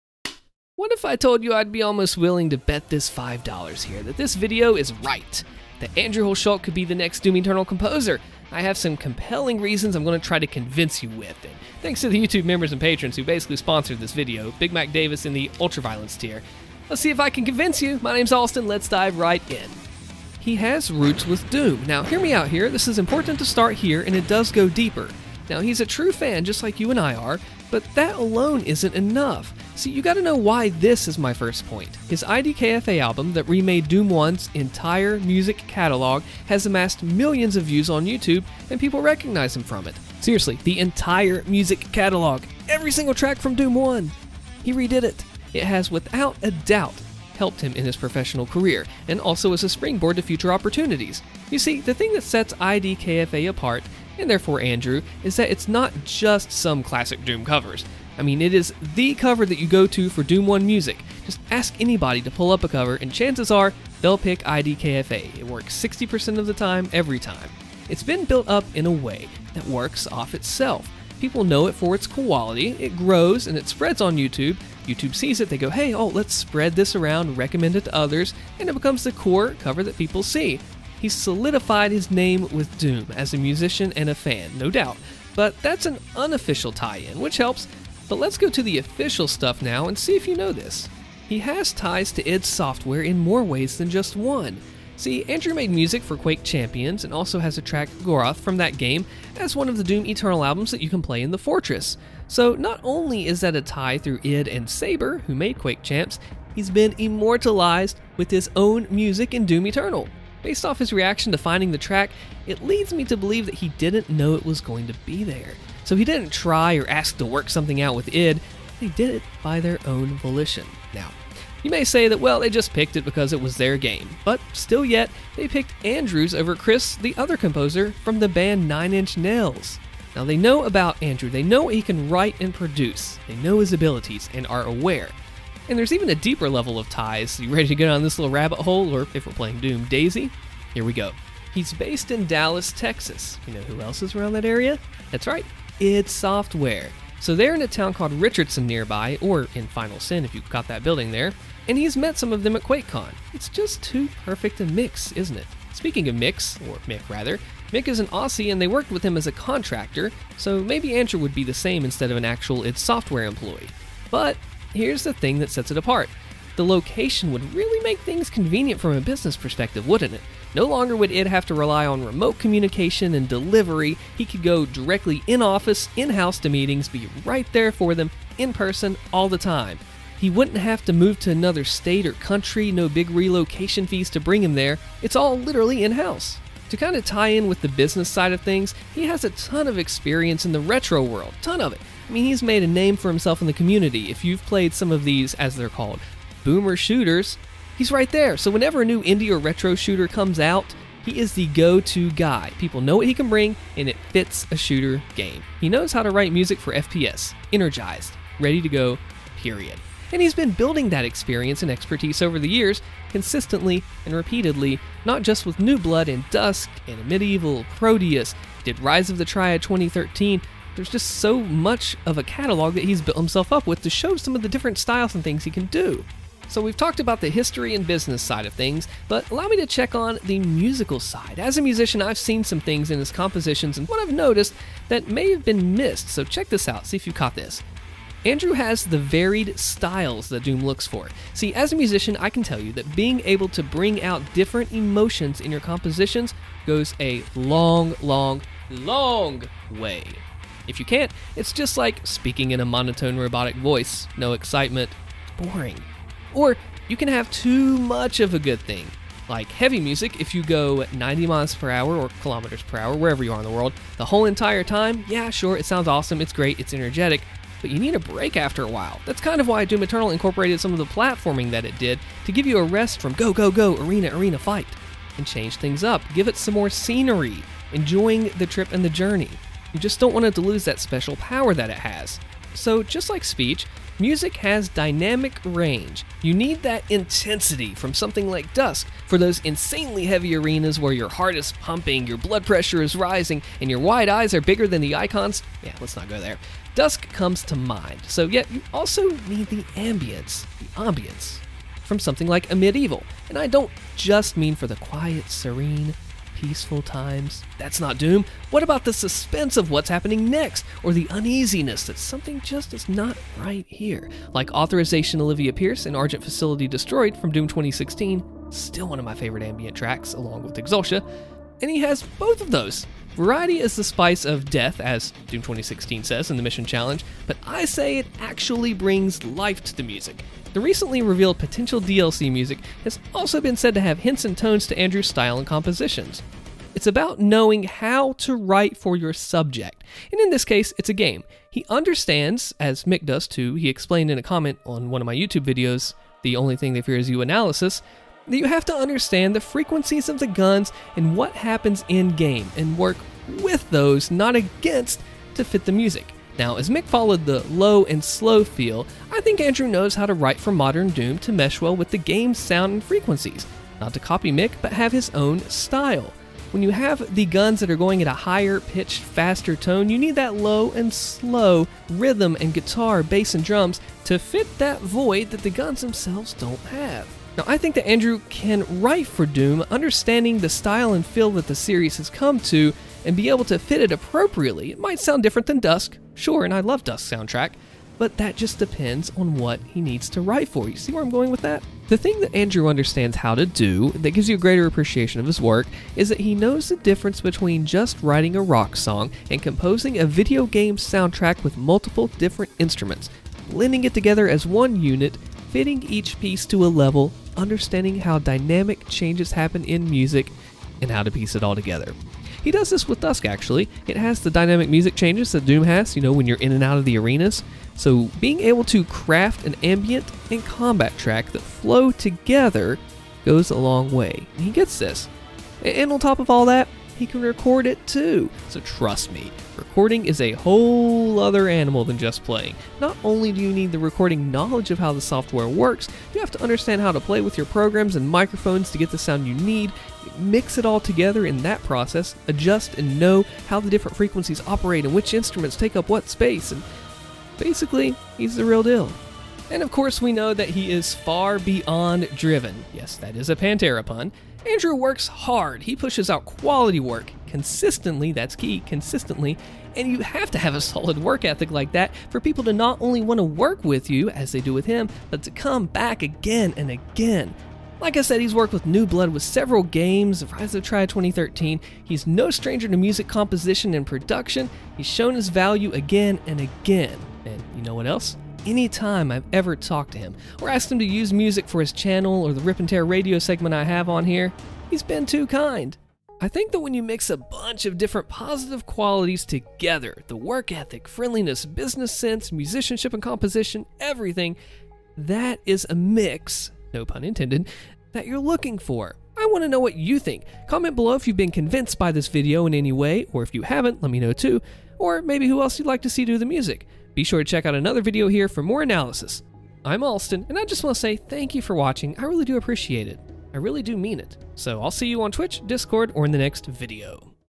Why I bet my $$$ Andrew Hulshult composes DOOM Eternal DLC
Here's why I'd bet money that Andrew Hulshult composes DOOM Eternal DLC! I think I can convince you too...
He has a ton of experience and is a huge Doom fan, and could make music that fits DOOM Eternal - this handful of reasons why Andrew Hulshult could compose for DOOM Eternal's DLC explains his business and music composition skills, from the perspective of a fellow musician.